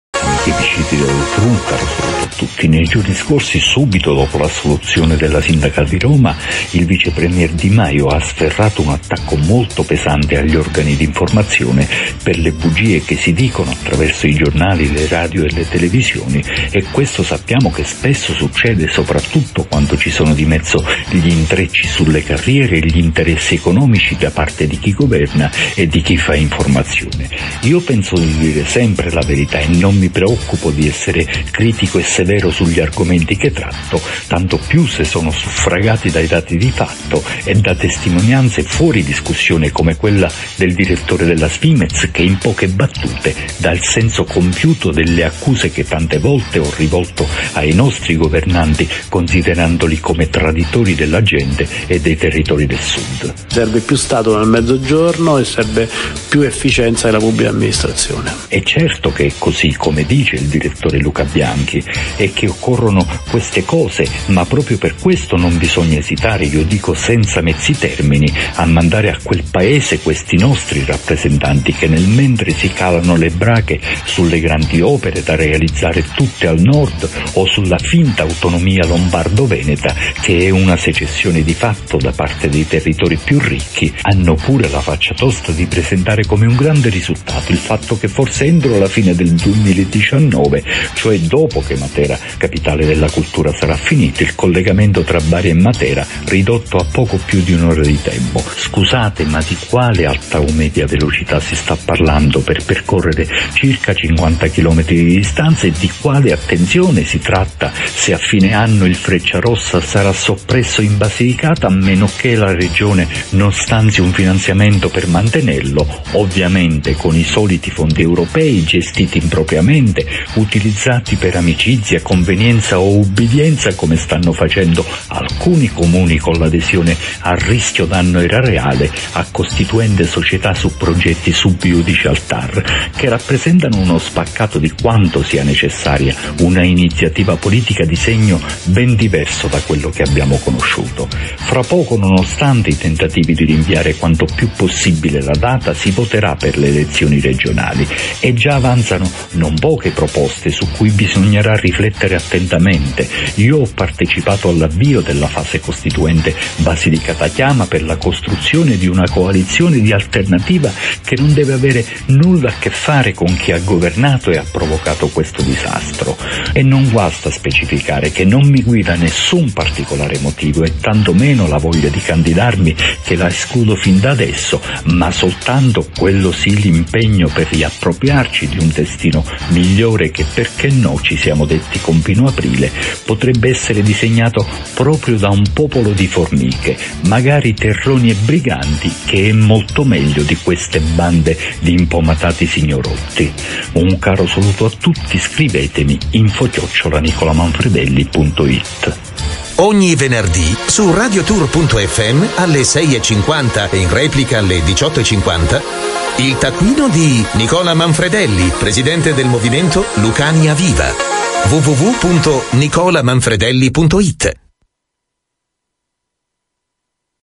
Radiotour | il Taccuino di Nicola Manfredelli del 16 novembre 2018
LO DICE IL DIRETTORE DELLA SVIMEZ | PRIMA SI CALANO LE BRACHE E POI ESULTANO SUL NULLA | UN POPOLO DI FORMICHE, MAGARI BRIGANTI E TERRONI, ANZICHE’ BANDE DI IMPOMATATI SIGNOROTTI | il Taccuino di Nicola Manfredelli su Radiotour